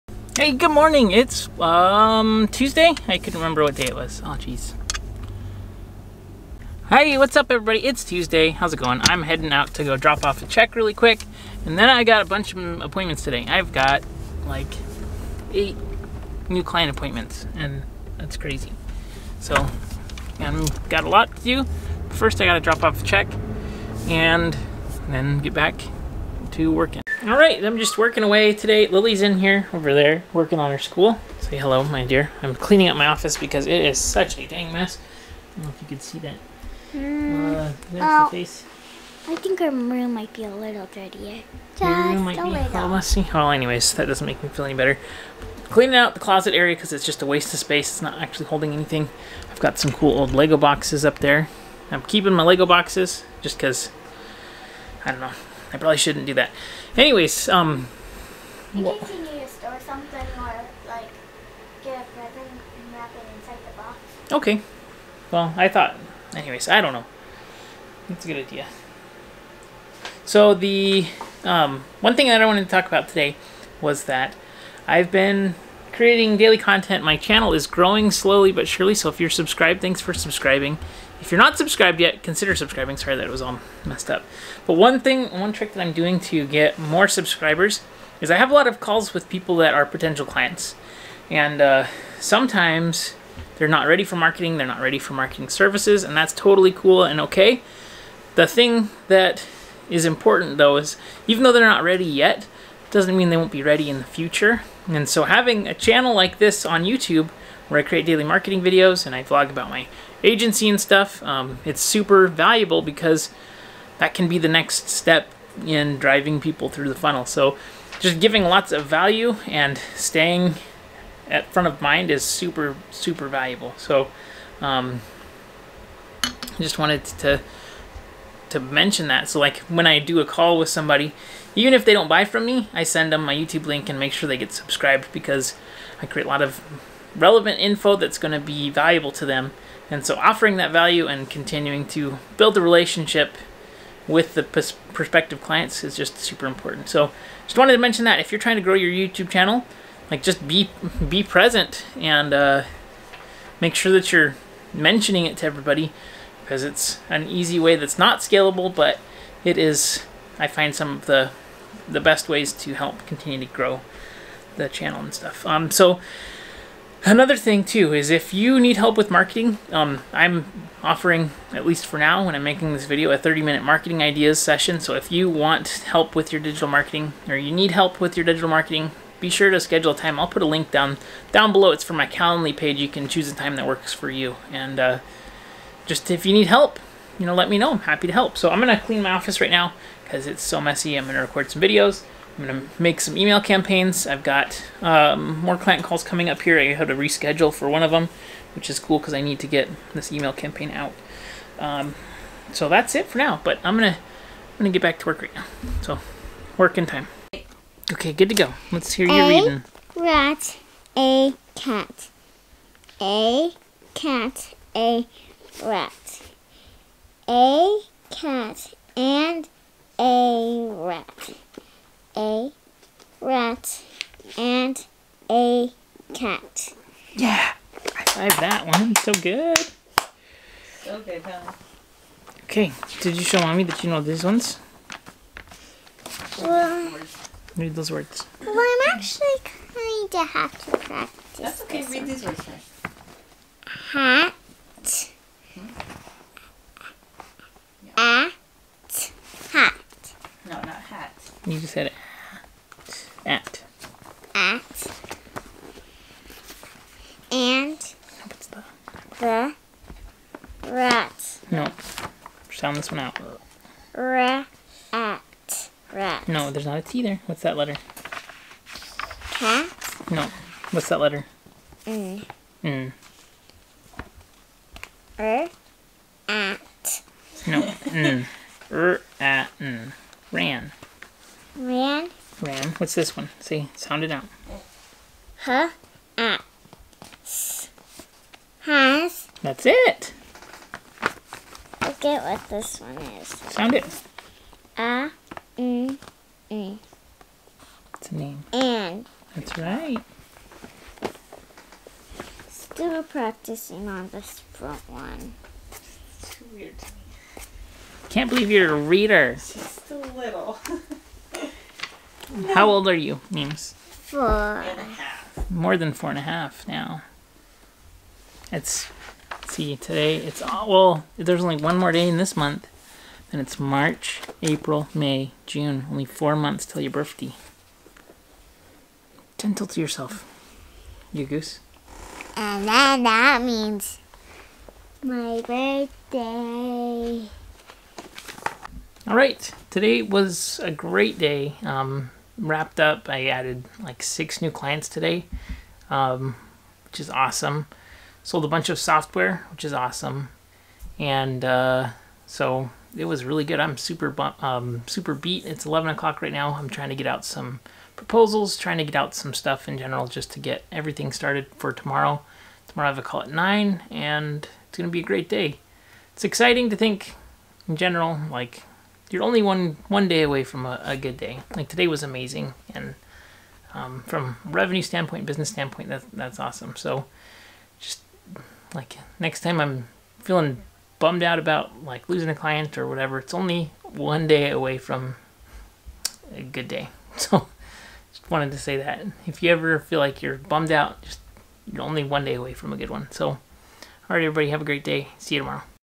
VLOG How I use Youtube To Keep In Touch With Agency Prospects
Today's vlog is filled with work and I talk about how I use my Youtube channel to keep in touch with prospective clients even if I don't close them or they aren't ready for client services.